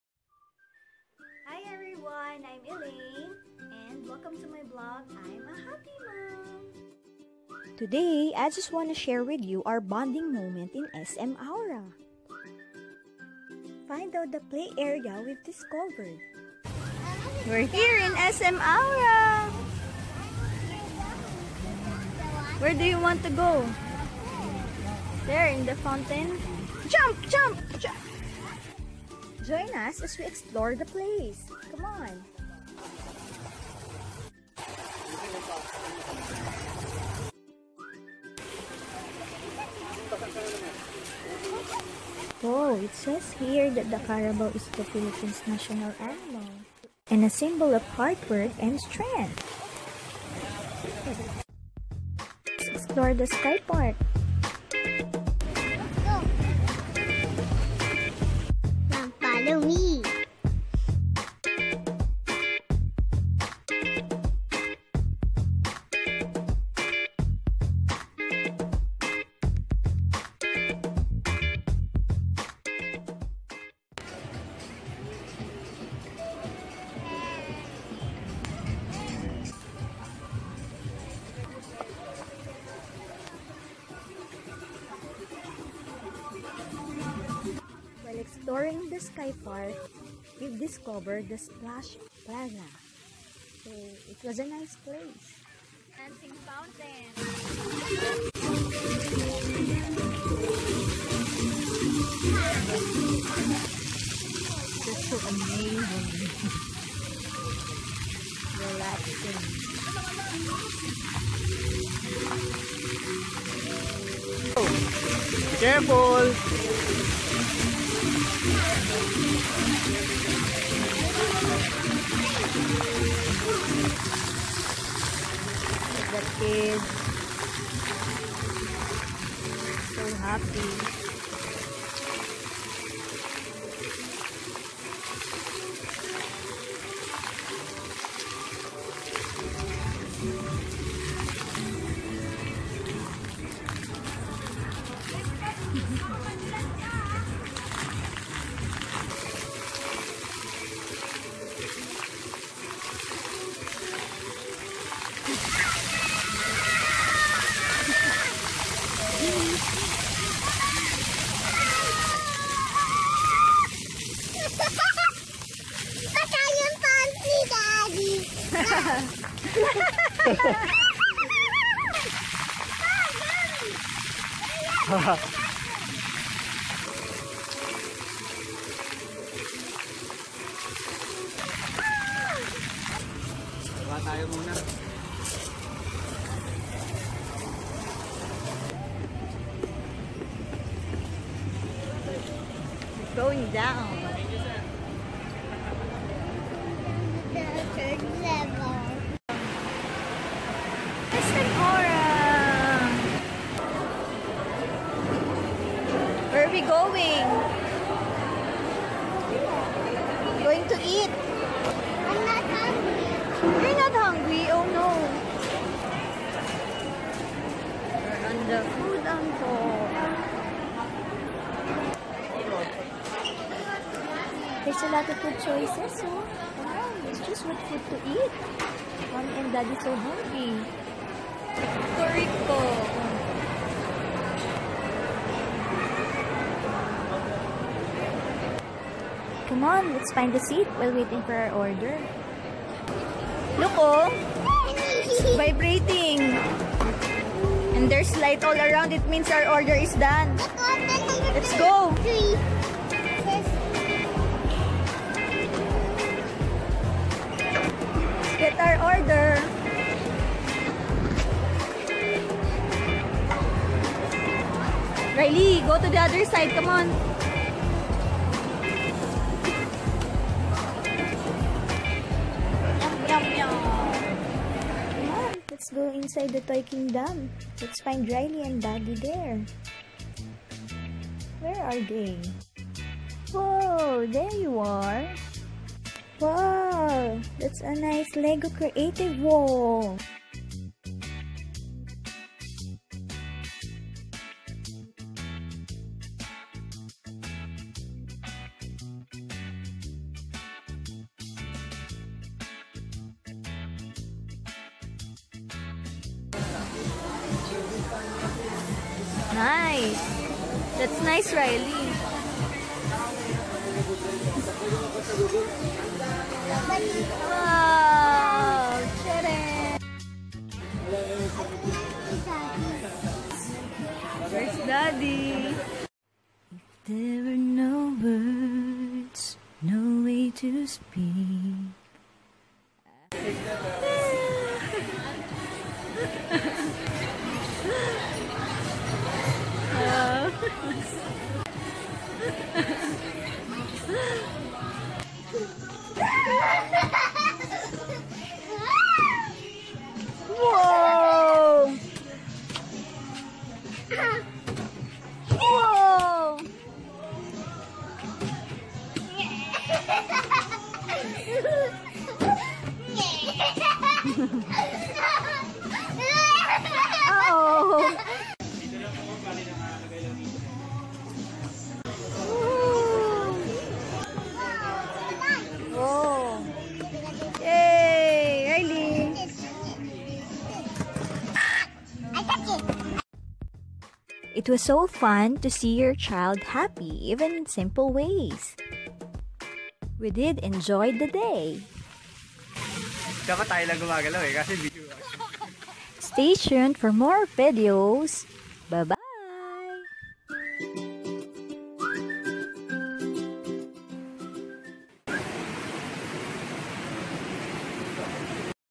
Playtime bonding at NO COST in SM Aura|| Family Bonding on VALENTINES DAY|| I'm a Happy Mom PH
We had a great time playing in SM Aura together with our son. We didn't spend too much in this bonding day. Just for our food..